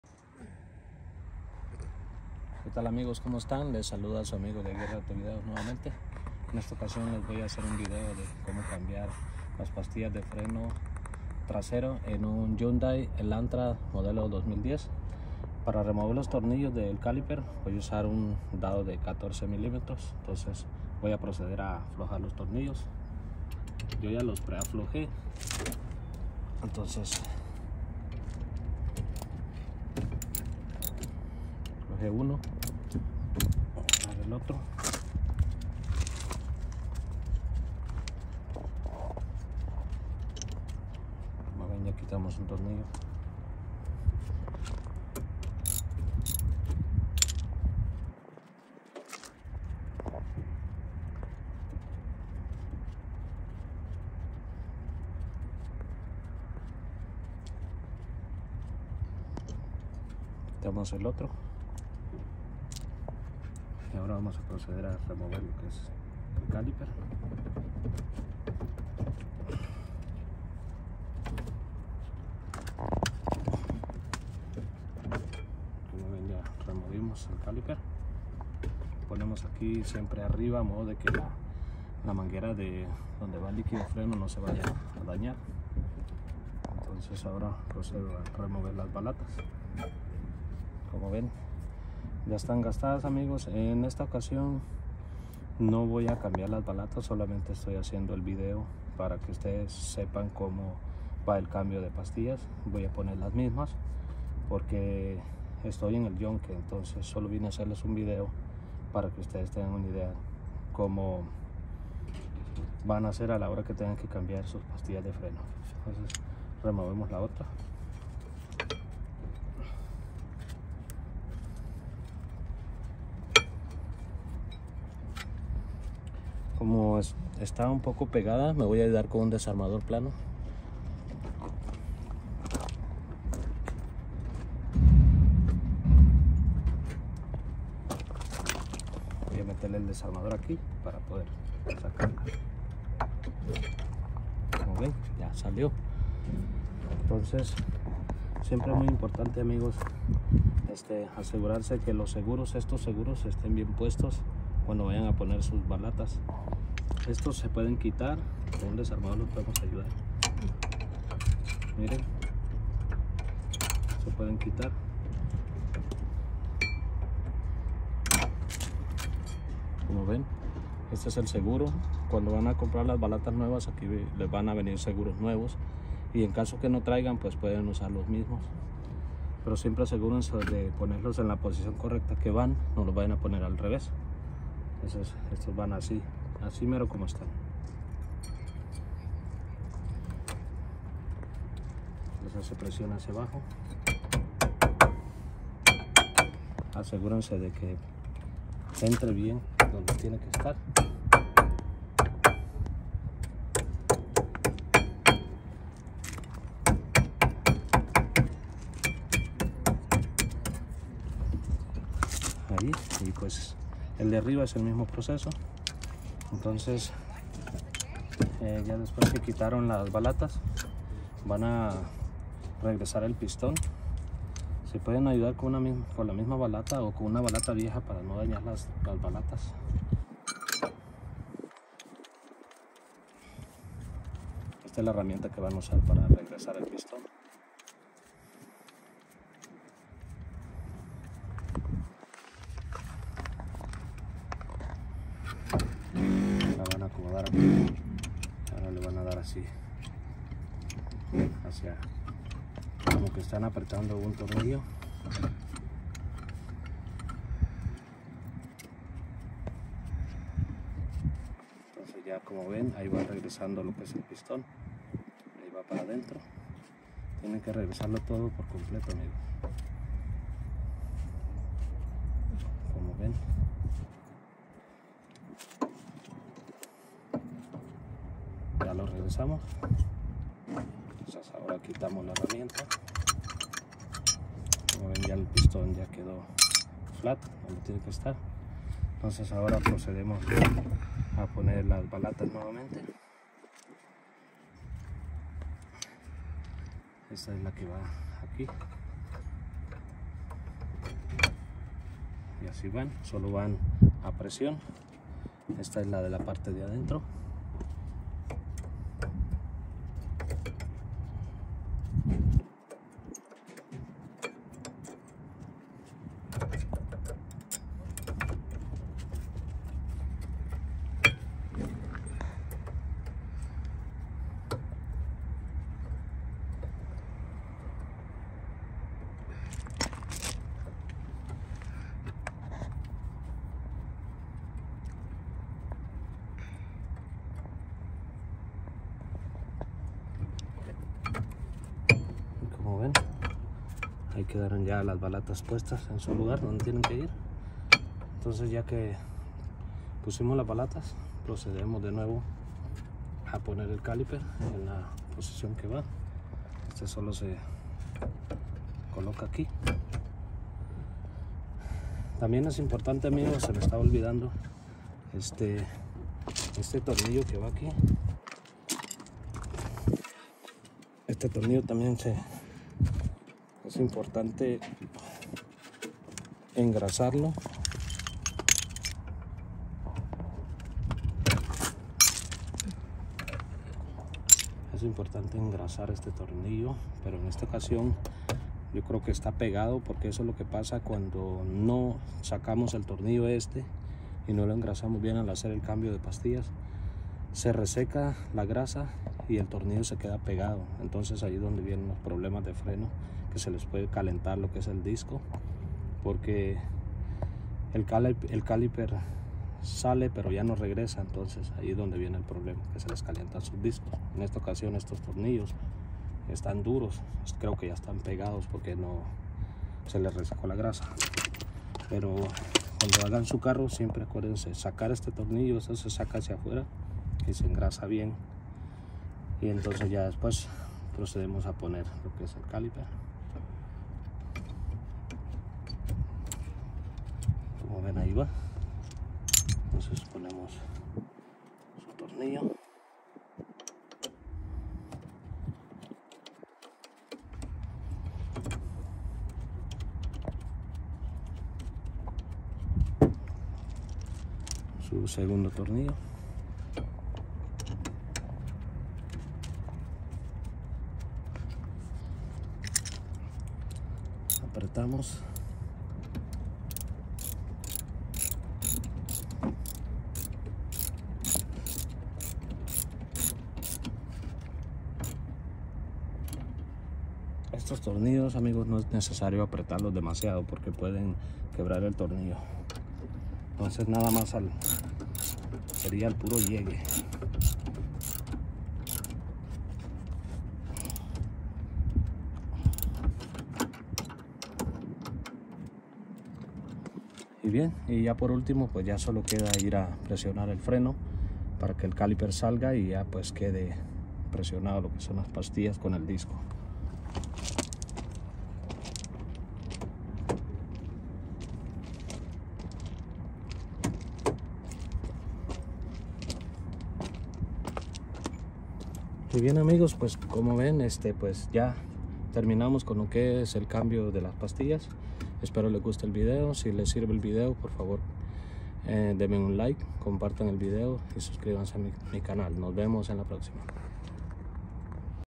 Cómo cambiar las pastillas o balatas de freno trasero a Hyundai Elantra hatchback 2010
Espero les guste y les sirva el video amigos y amigas gracias por el apoyo